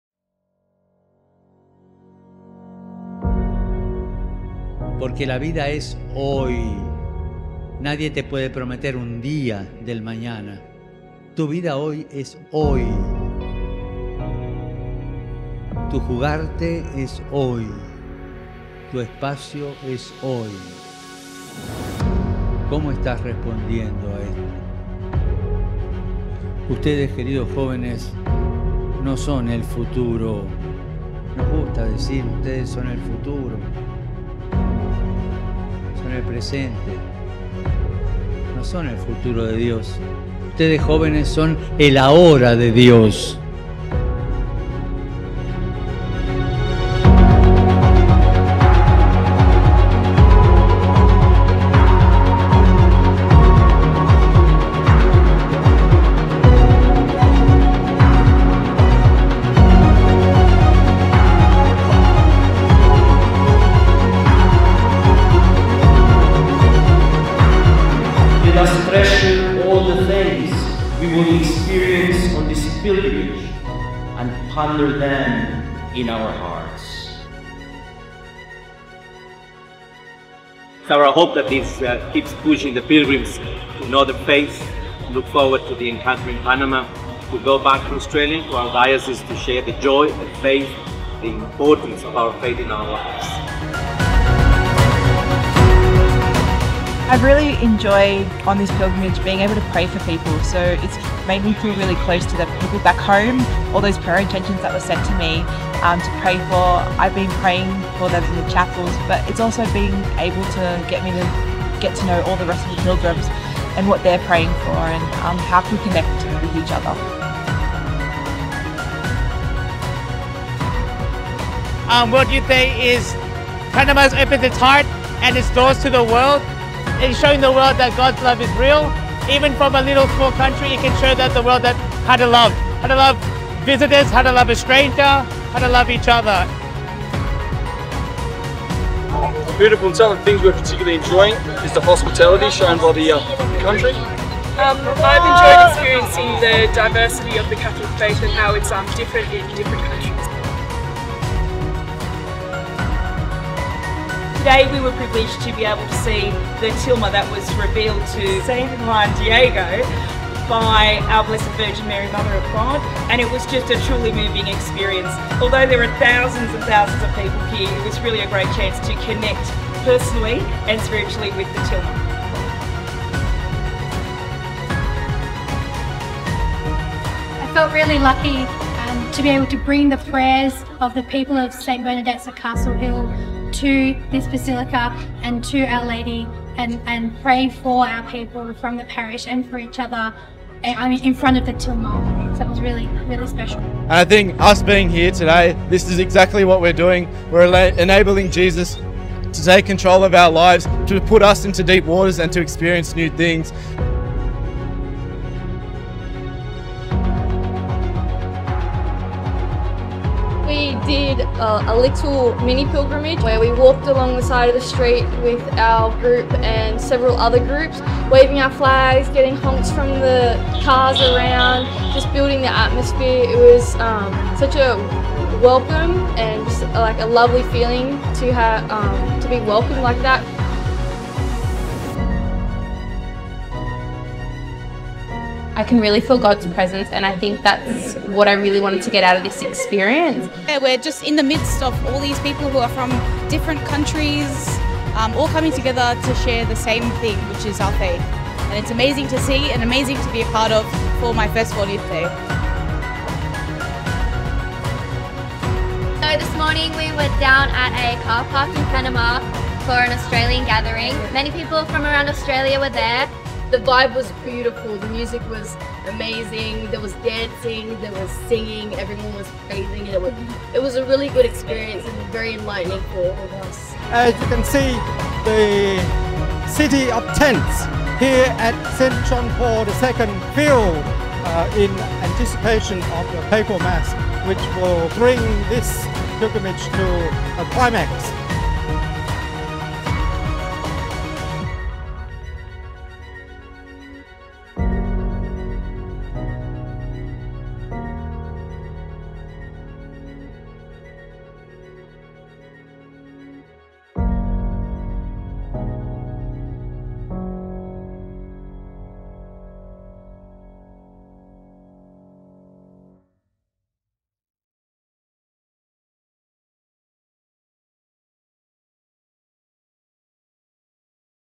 WYD23 Promotional Film - Diocese of Parramatta
World Youth Day is a powerful encounter between young people, Jesus Christ, Pope Francis, cultures of the world and the universal Church. The Diocese of Parramatta, in Western Sydney, Australia will be leading a pilgrimage to Lisbon, Portugal in July 2023.